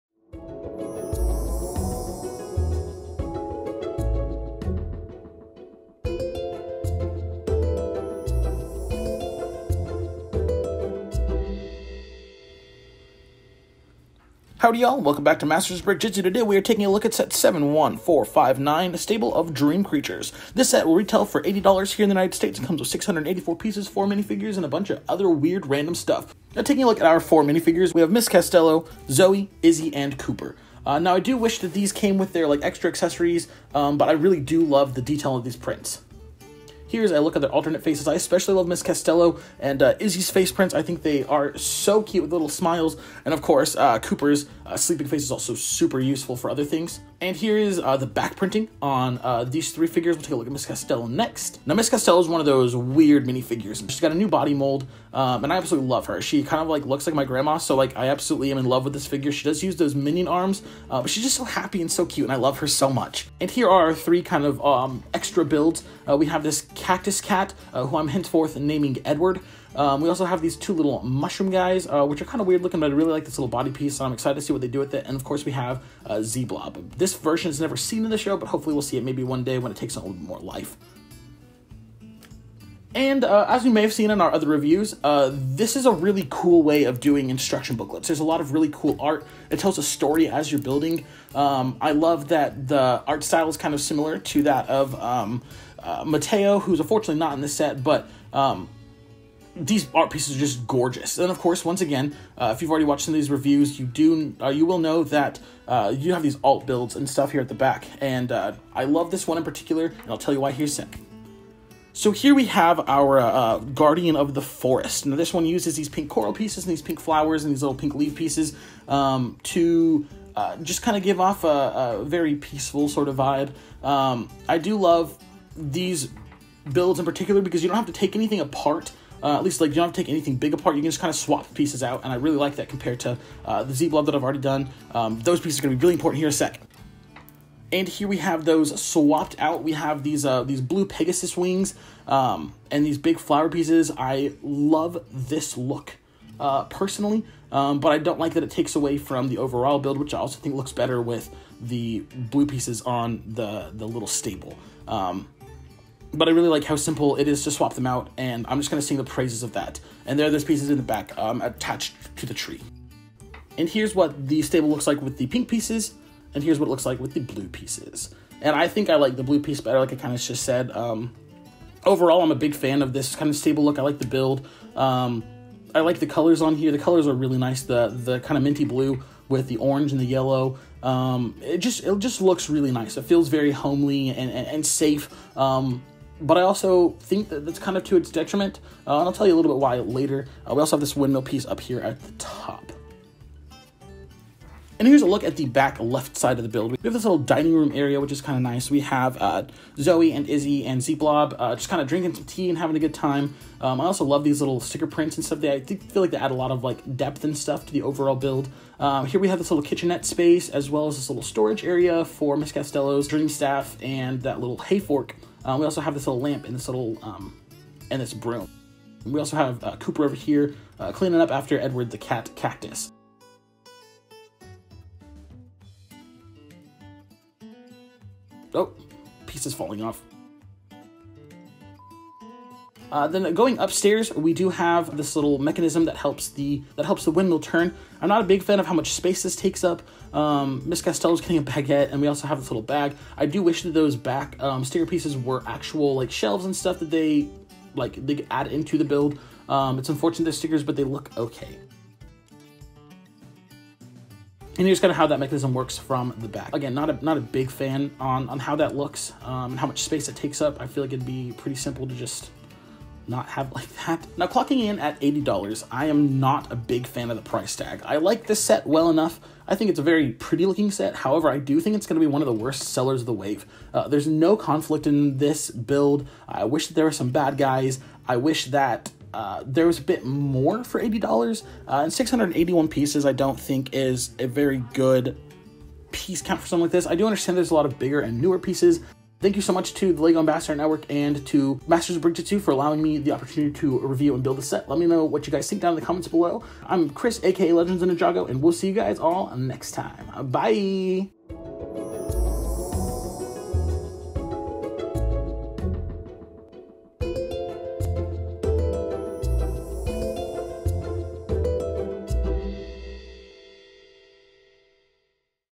The Worst LEGO DreamZzz set?! | 71459 Stable of Dream Creatures | Early Review (2023)
Chris aka Legends of Ninjago is out in stables, but is this set worth the price? Will this warms shelves? Is a pizza a sandwich?!